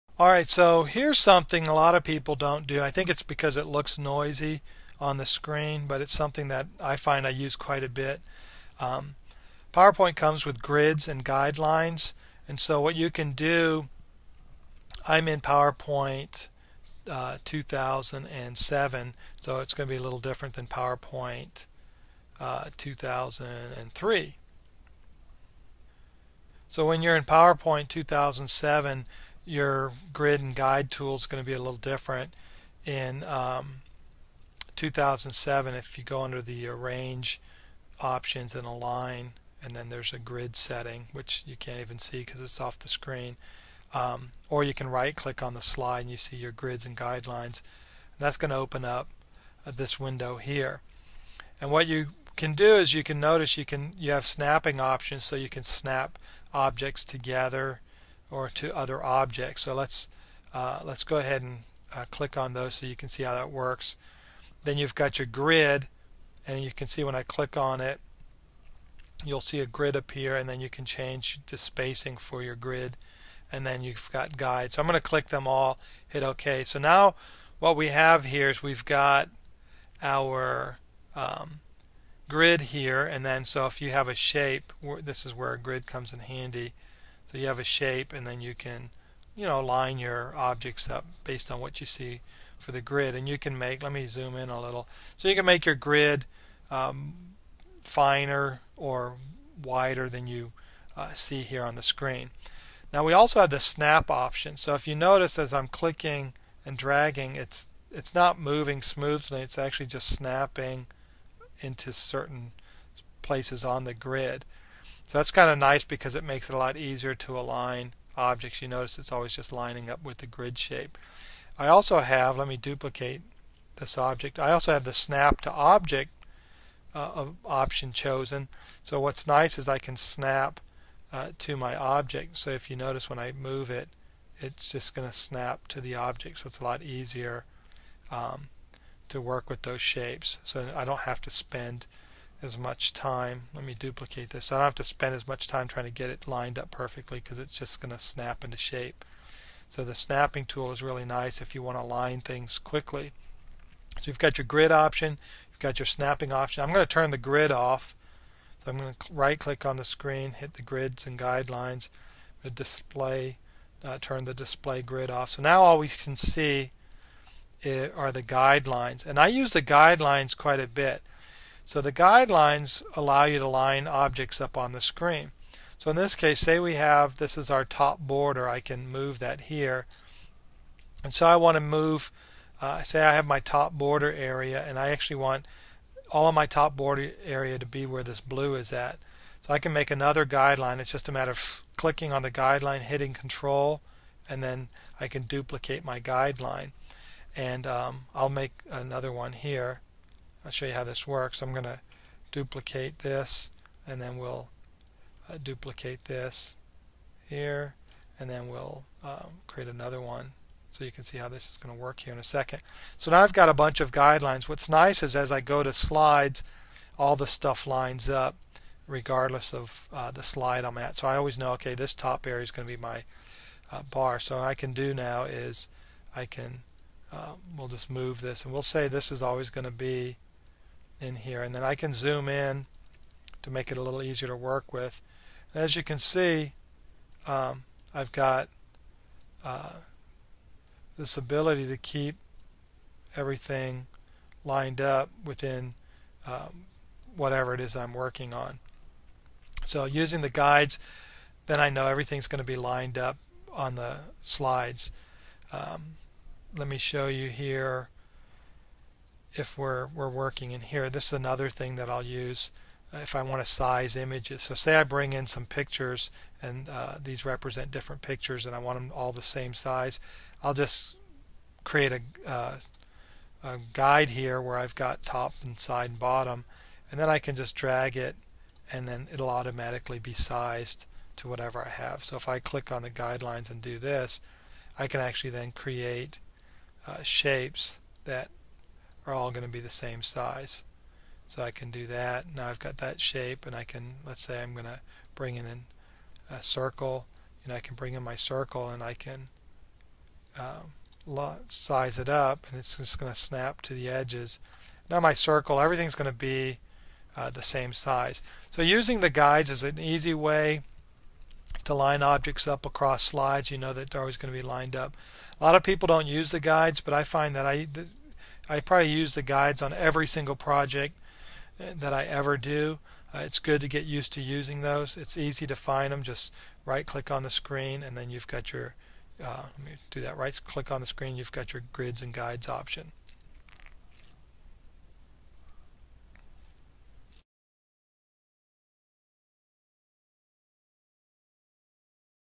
Using Grids & Guides in PowerPoint
Learn to use PowerPoint's grids and guides to align objects when building elearning courses.